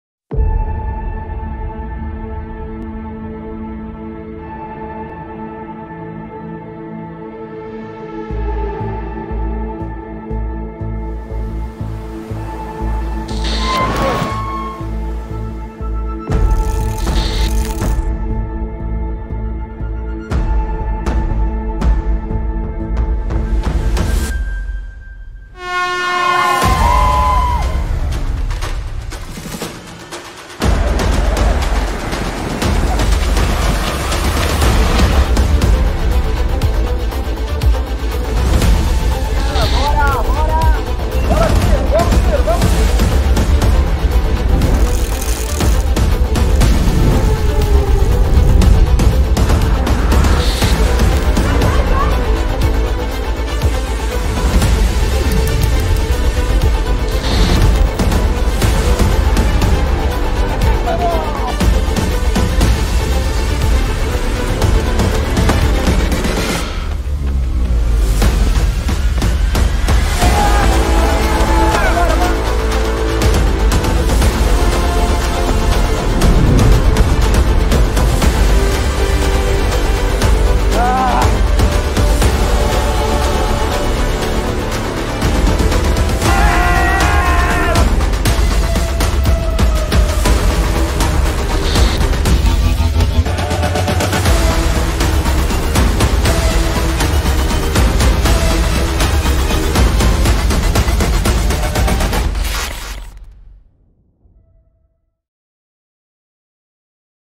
Triathlon Internacional de Santos
Com um percurso desafiador de 1,5KM de natação, 38KM de ciclismo e 10KM de corrida. Assim foi a edição de 2023 do Triathlon Internacional de Santos.

Confira!